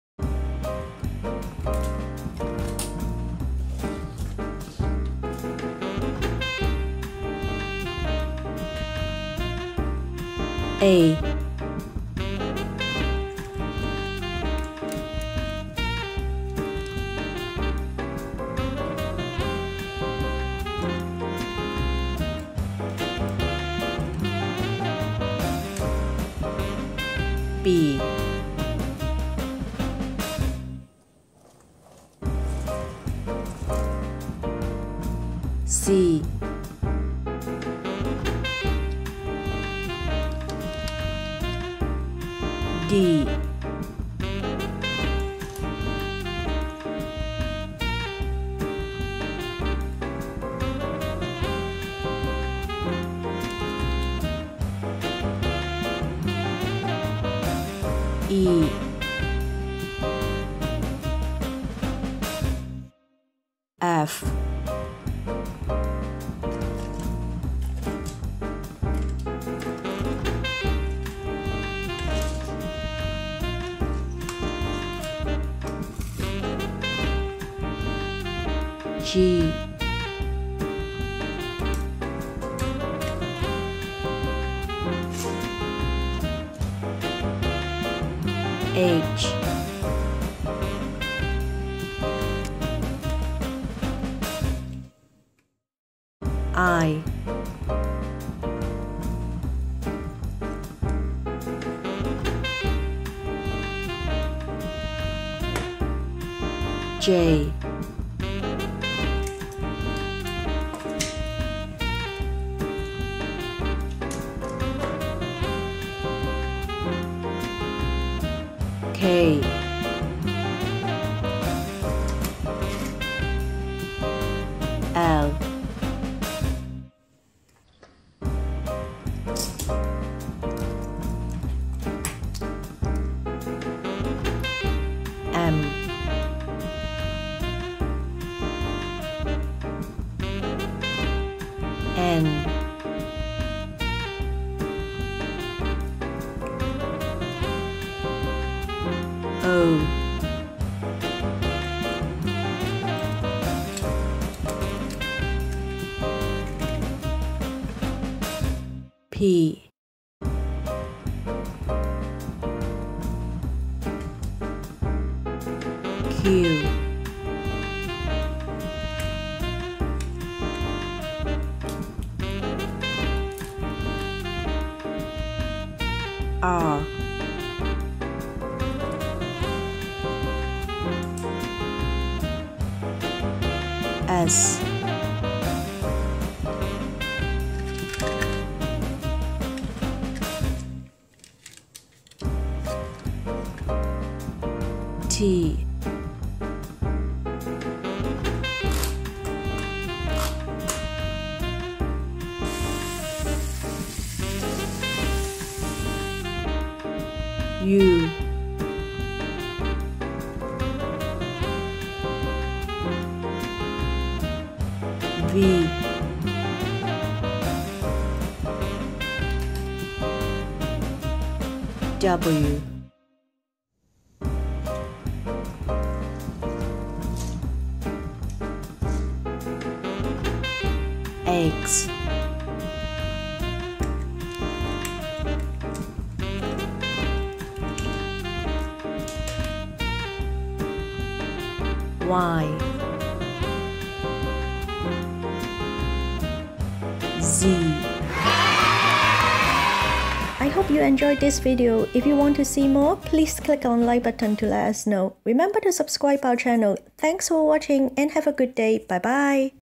Learning letters with Street Vehicles for kids with tomica siku (Upper Case)
Teaching Upper Case letters to kids with different special vehicles tomica siku VooV ブーブ . This video covers special vehicles which include fire truck, truck, thomas train, car, fuel truck, hydraulic jackhammer

Kids can learn Upper Case letters with Street Vehicles in this video. It is simple and fun!

- SHARE it with friends on Facebook, Google+, Pinterest, etc.